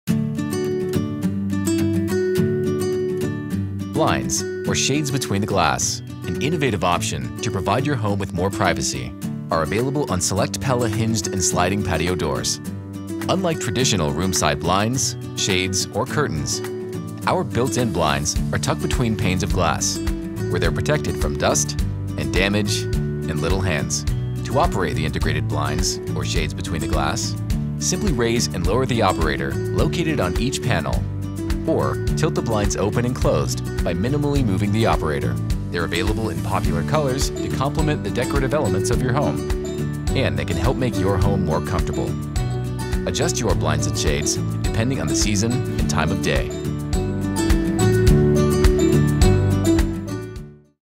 Benefits of Patio Doors with Blinds-Between-the-Glass
Add privacy to your home with blinds- and shades-between-the-glass. Tucked away between panes of insulating glass on French doors and sliding glass doors, blinds-between-the-glass and shades-between-the-glass are protected from dust and damage. Explore Pella’s offering of built-in blinds to find the right choice for your home.